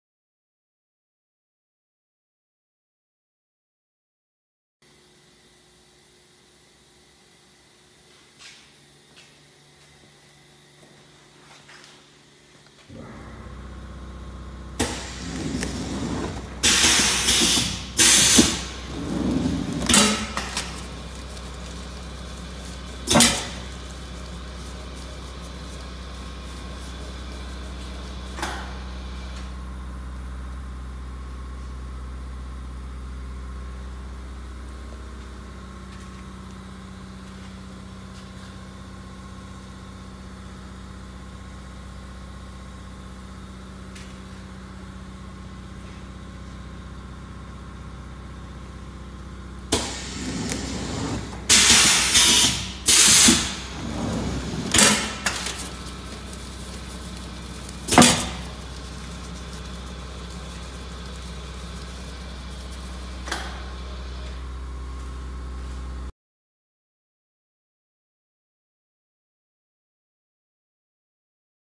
473-205: Tecnovac traysealer
Tecnovac mobile traysealer, dye: (1x) 268 mm x 233 mm, Busch vacuum pump, total dimensions: 1200 mm x 1050 mm x 1550 mm.